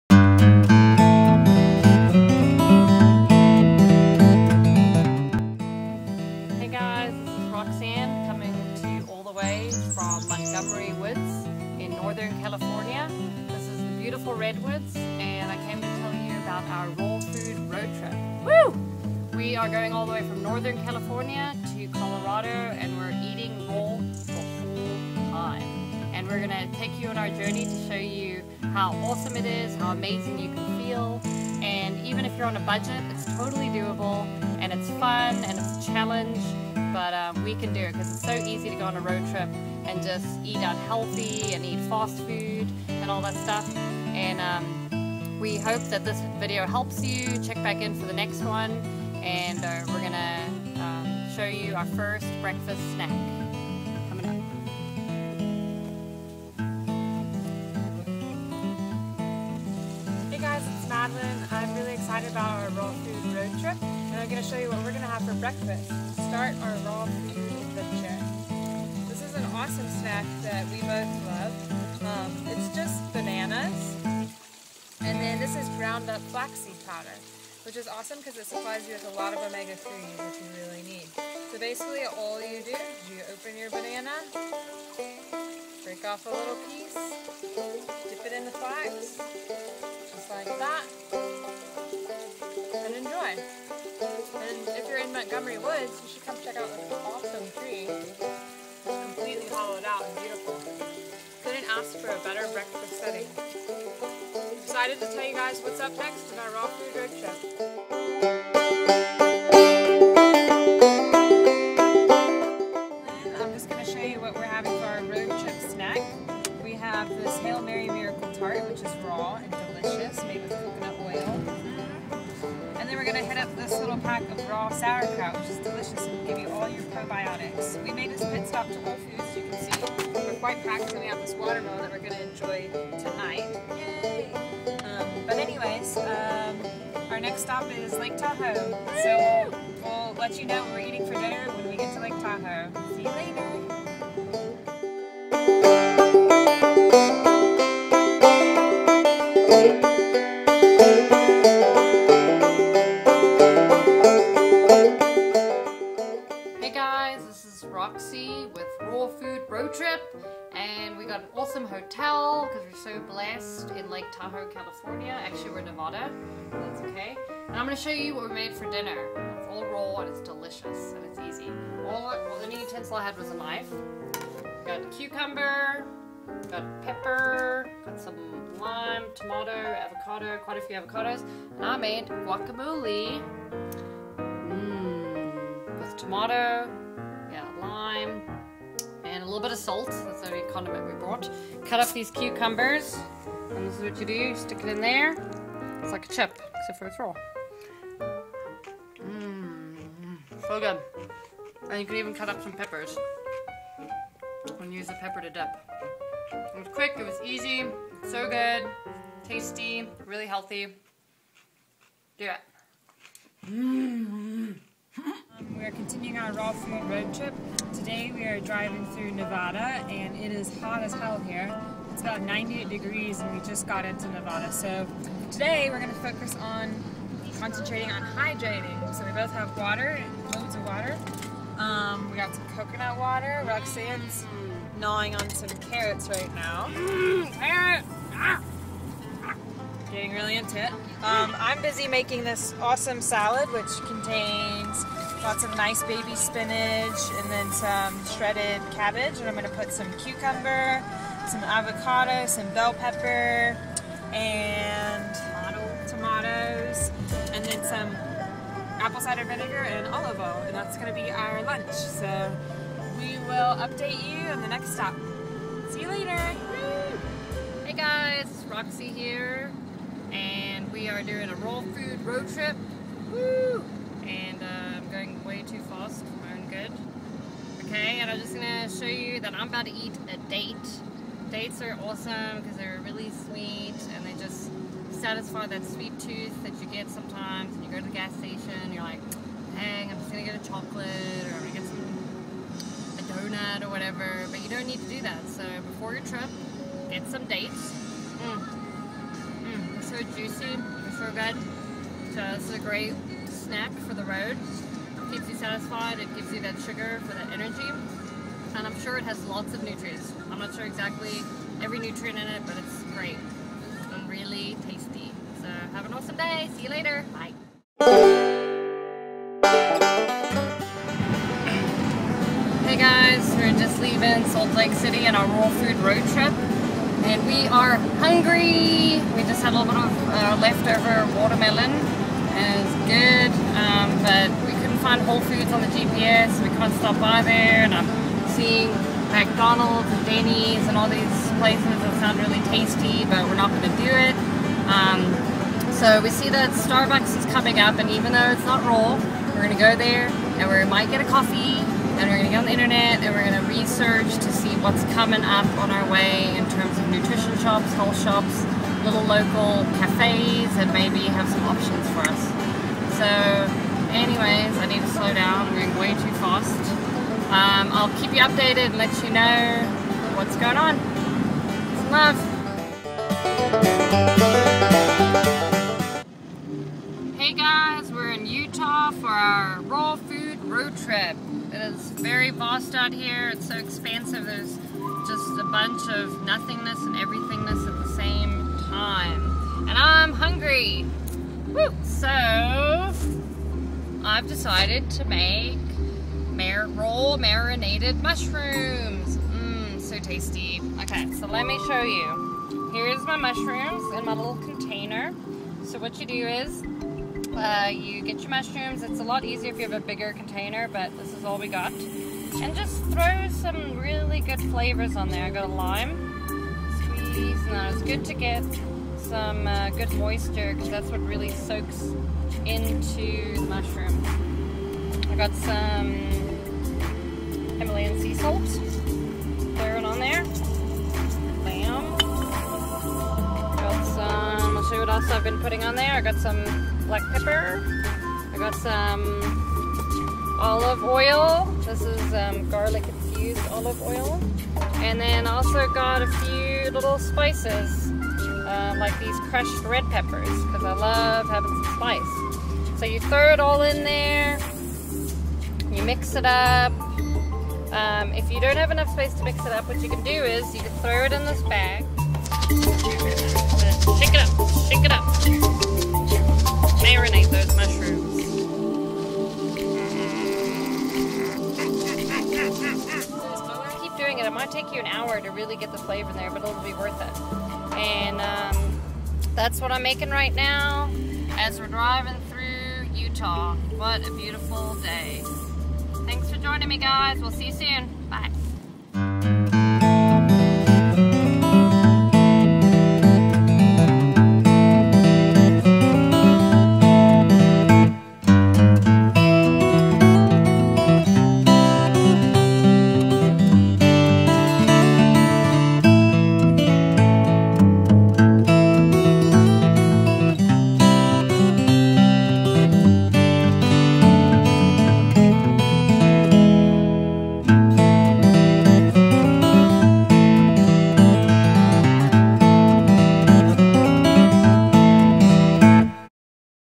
Raw - Food Road Trip - 100% raw for the whole journey - You can do it too!
We traveled from Mendocino California to Vail Co and ate only raw food the whole 3 day trip!  Eating raw is one of the most amazing things you can do for your mind, body and soul.  Your will feel the raw energy flowing through your blood.  When you eat food in its raw state, it keeps the needed enzymes that help your body digest and receive all the nutrients.

The first step is to plan your raw food road trip by planning your stops ahead of time and researching where you are going to pick up your raw meals.

  We had so much fun and felt awesome when we arrived to our new home!